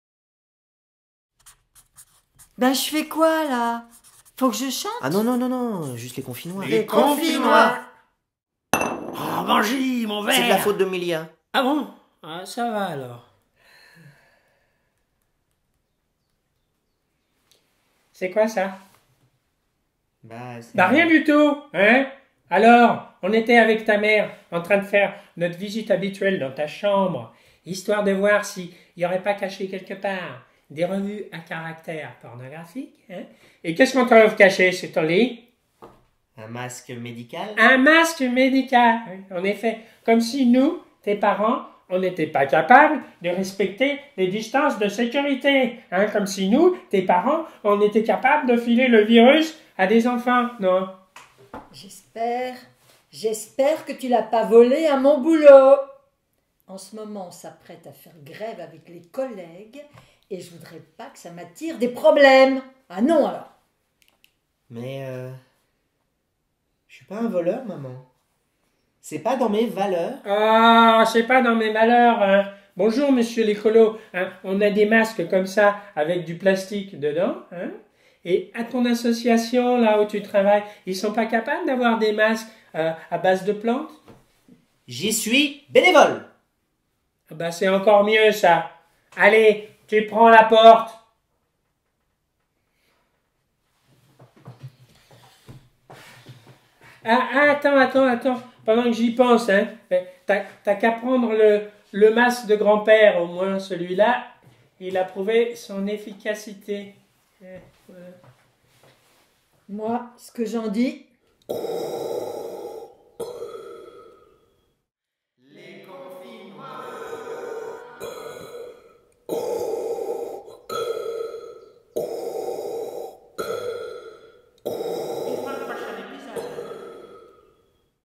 Les Confinois - Épisode 3
La série du confinement de Riquet, Milia et Benji, une famille bien de chez nous 🇫🇷
Écrit & Réalisé par Emilia, Eric Turlot et Numéro B
Avec Emilia, Eric Turlot et Numéro B
Montage & Son : Numéro B